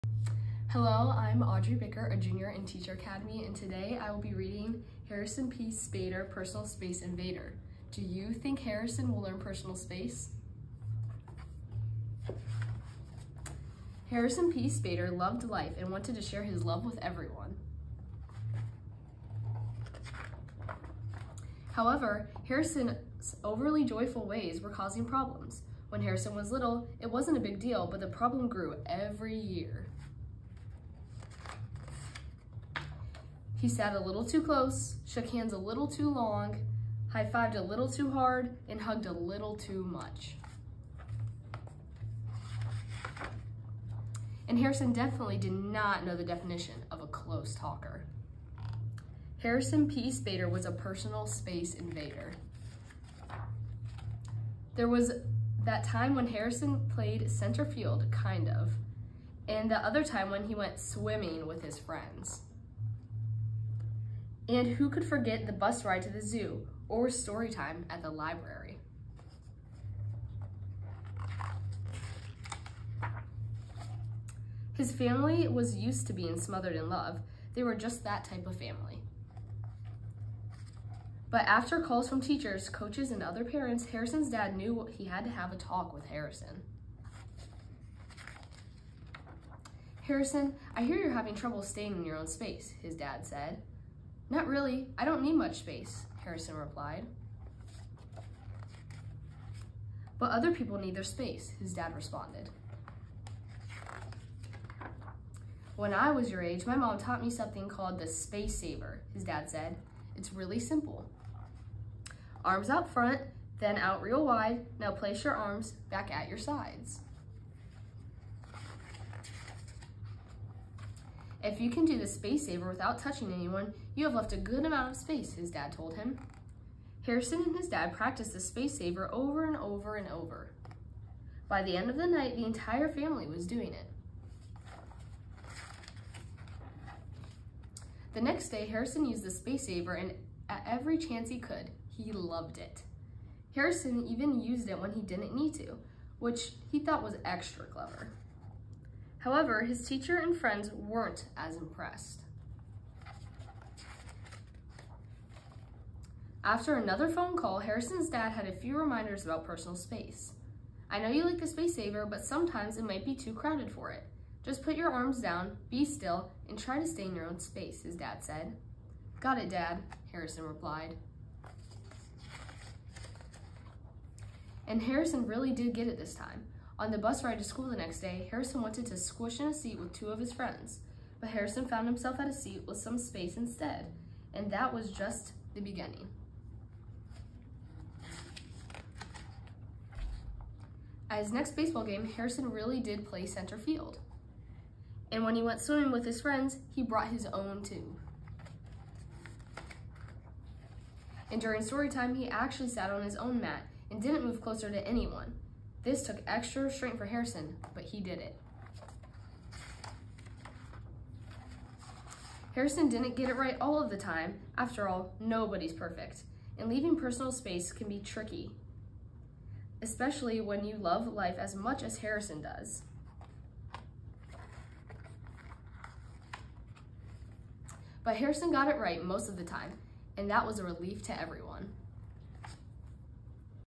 Harrison P. Spader: Personal Space Invader
In this video, a PHS Teacher Academy student will read the book Harrison P. Spader: Personal Space Invader written by Christianne C. Jones and illustrated by Cale Atkinson.